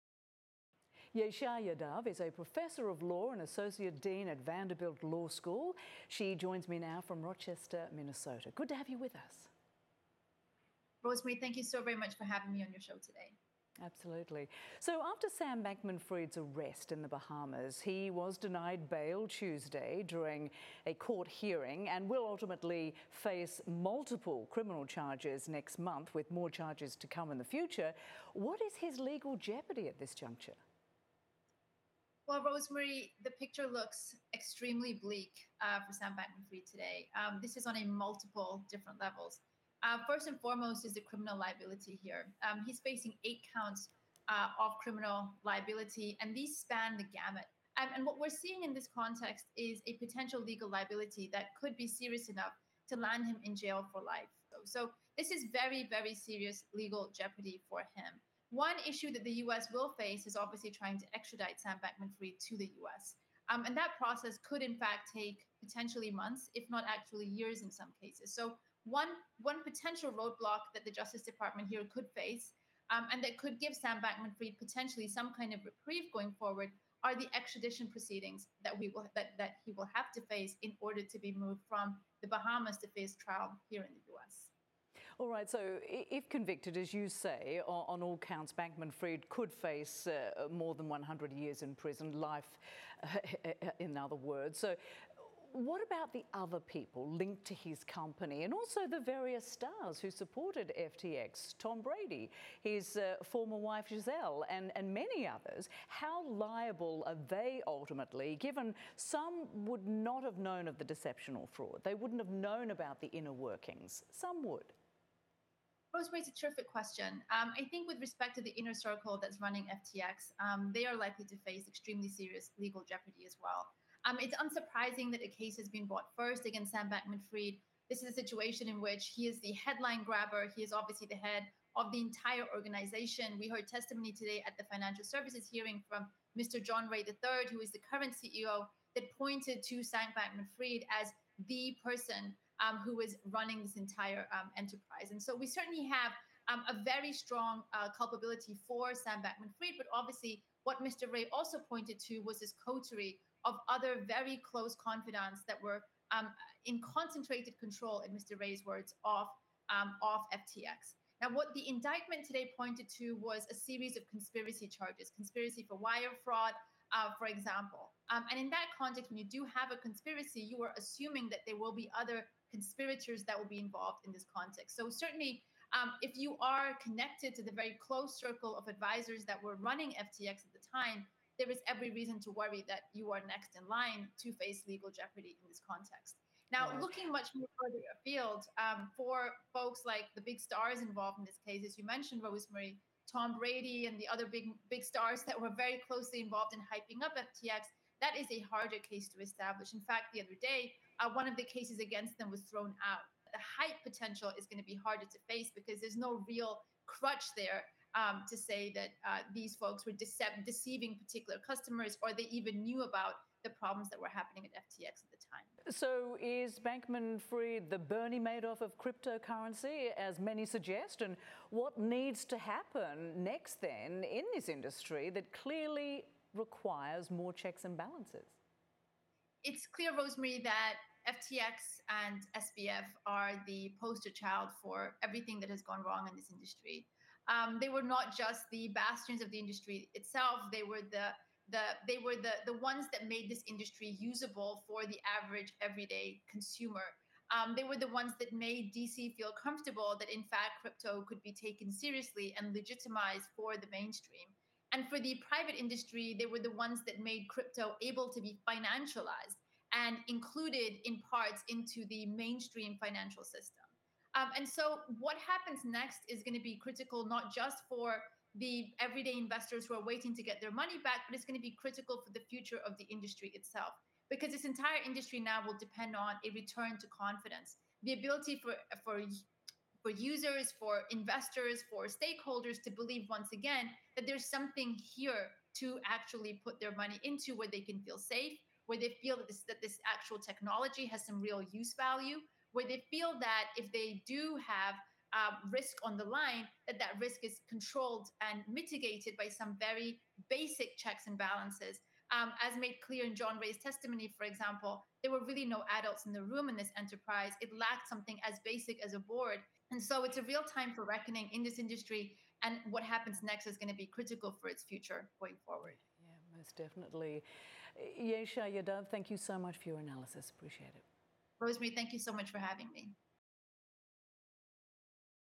Securities law expert Yesha Yadav interviewed by Rosemary Church of CNN International, Dec. 13, 202
Yesha Yadav, professor of law, discusses the arrest of Sam Bankman-Fried, founder of the cryptocurrency exchange FTX, for wire fraud and conspiracy with CNN International anchor Rosemary Church.